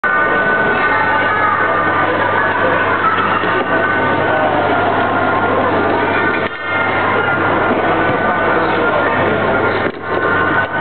Jugando con discos sobre una plataforma giratoria
Varias personas jugando en un experimento del Exploratorio del Museo de las Ciencias de Valencia, que lleva por nombre "Mesa con memoria". En realidad, se trata de una plataforma giratoria sobre la que un disco rodante tiene un comportamiento muy curioso. Más información en el vídeo "Disco rodando sobre una plataforma giratoria"